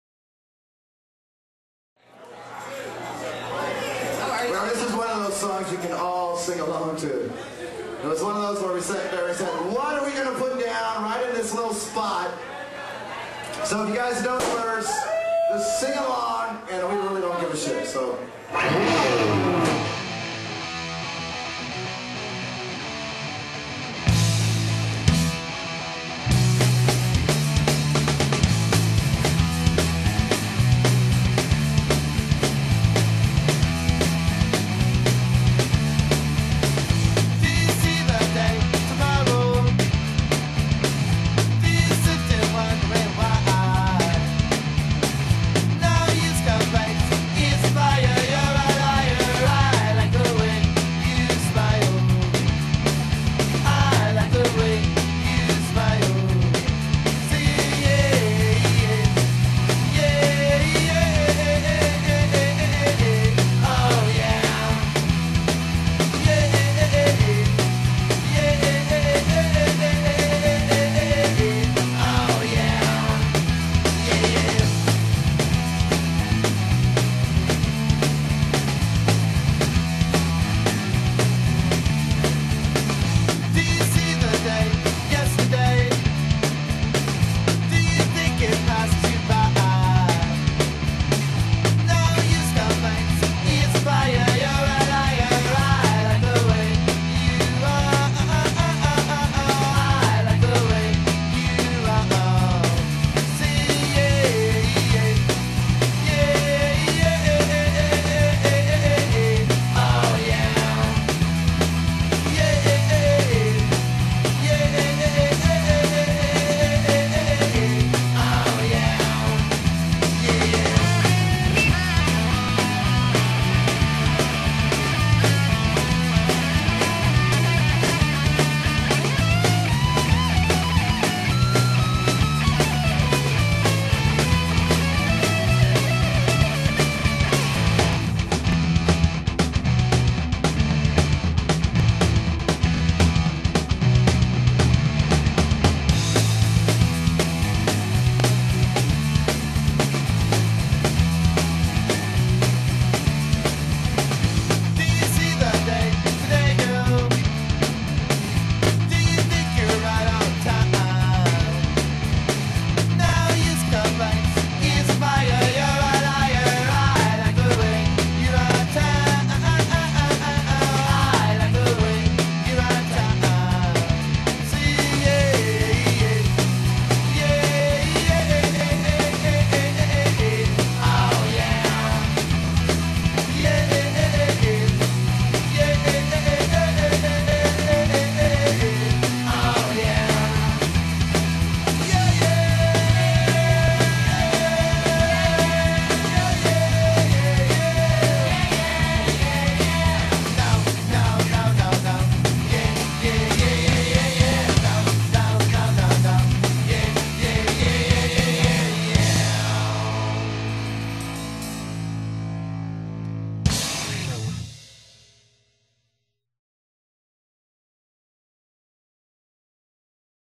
Lesbian Muff Divers (the band)
Lesbian Muff Divers play live in Seattle.